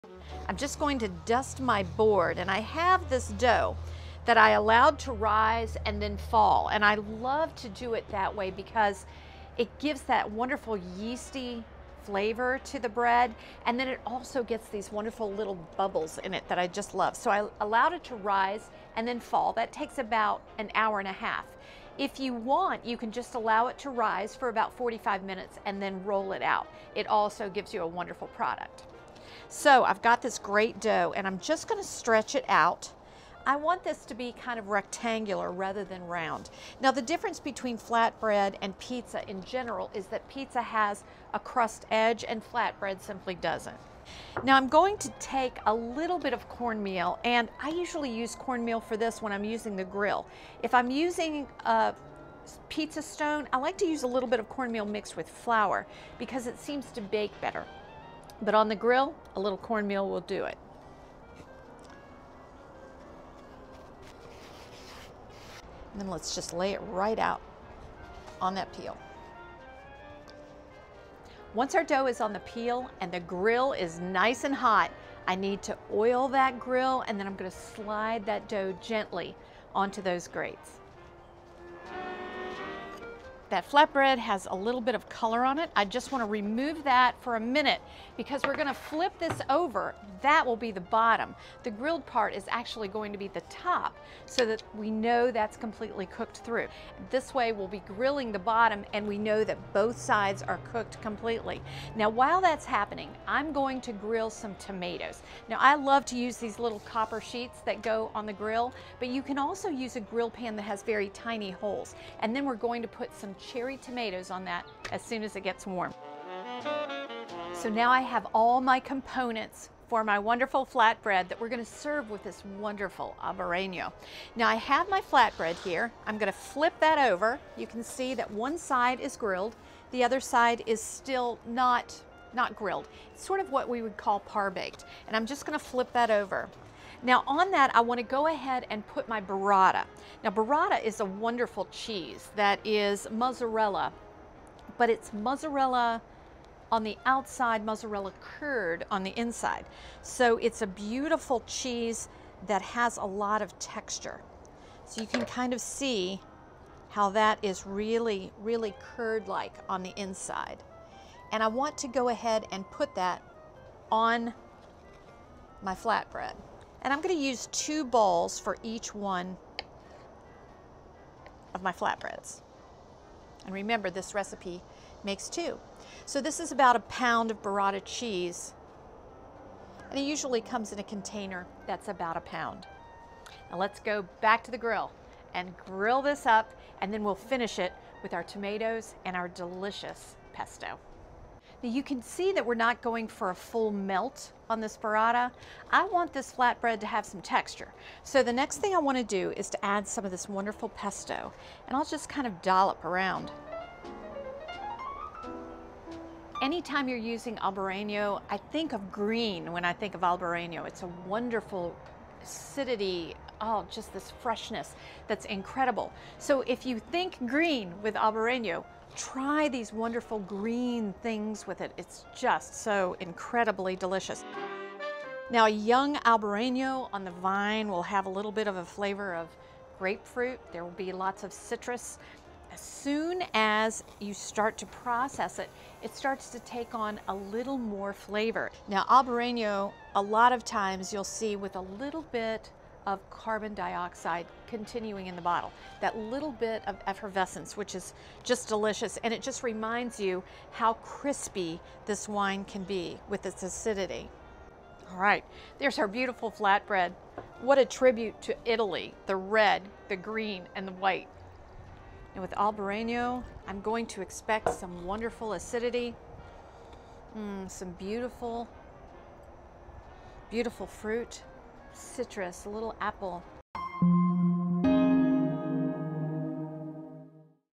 Easy To Make Flatbread With Burrata and Arugula Pesto
Flatbread is a hit for appetizers, lunch, or for a light dinner. The great thing about flatbread is that anything can top it. For this one, the creaminess of the burrata is paired with the grassiness of the arugula to highlight this varietal of grape. Albarino is always a light and refreshing wine that goes perfectly with this combination!